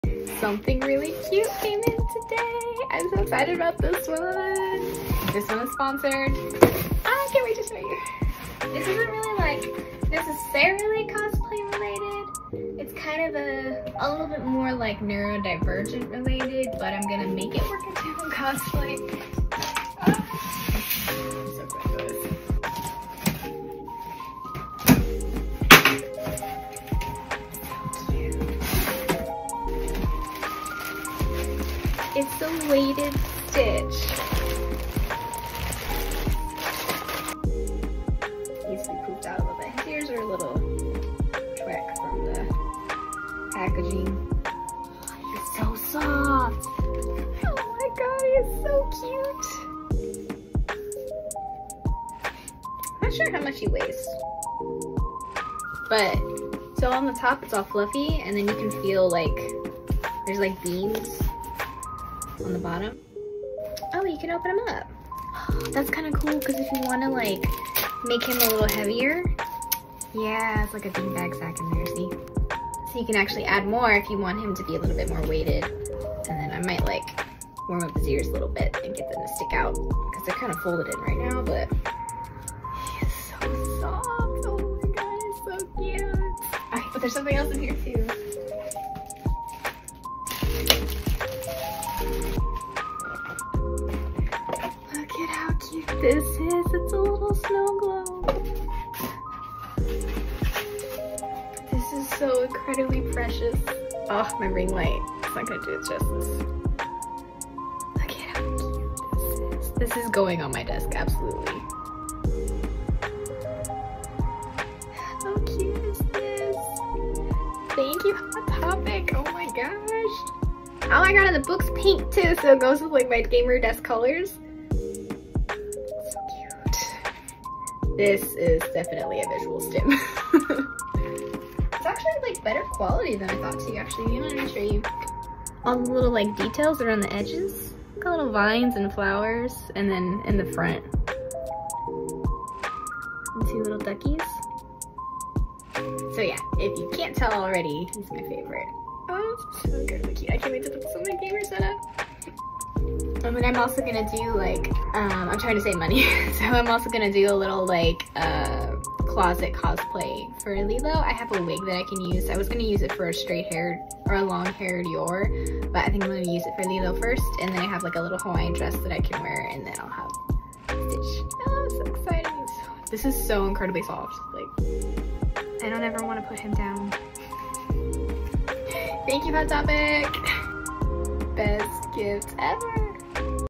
Hot Topic Unboxing!
I’m obsessed with this stitch merch! Thank you HotTopic 😭💕